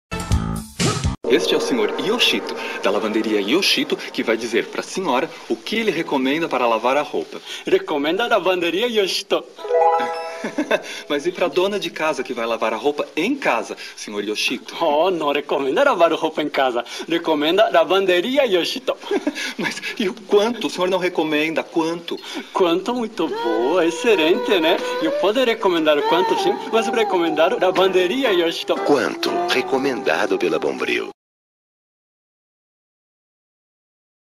MEME POLÍTICO (Yoshito não recomenda perdoar a dívida de R$ 1 bilhão, Bolsonaro!) - Exxocet 1127
Contexto: Bancada evangélica exibe força no governo de Bolsonaro.
Período: 07/09/2020
Local: Brasília / DF